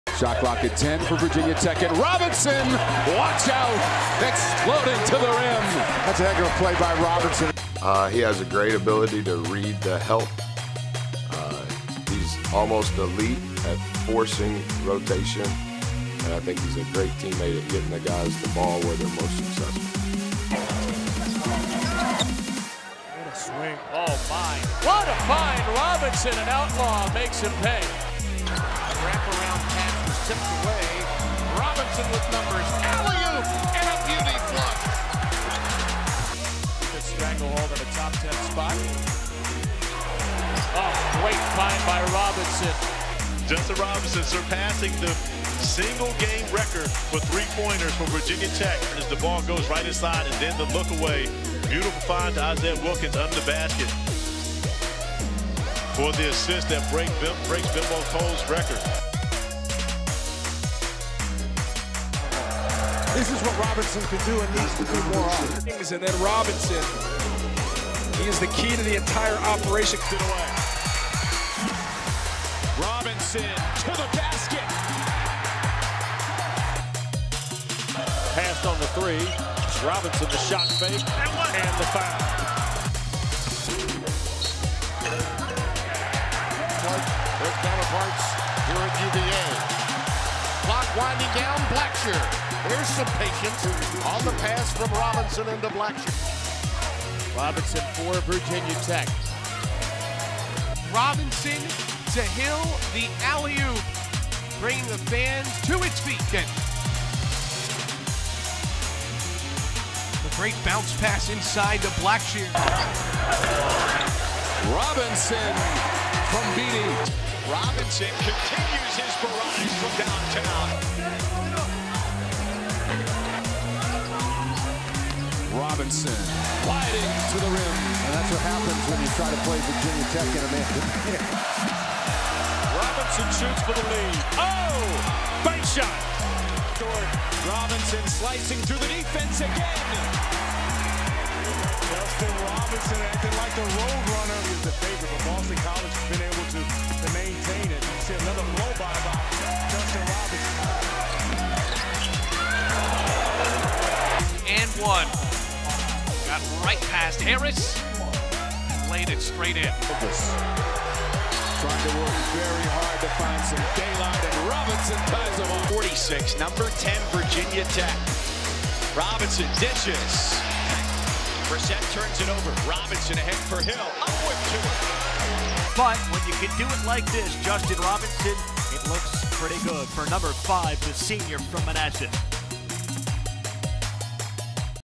Justin Robinson Official Highlights | Virginia Tech Guard
Virginia Tech guard Justin Robinson has been one of the best players to come through the Hokies basketball program, and he finished off his college career with a solid senior year.  Robinson averaged 13.5 points, 5.0 assists, and 3.2 rebounds per game in the 2018-19 season.  Despite missing a big chunk of the ACC season, Justin Robinson still had plenty of highlights and made his impact felt on one of the best seasons in Virginia Tech's history.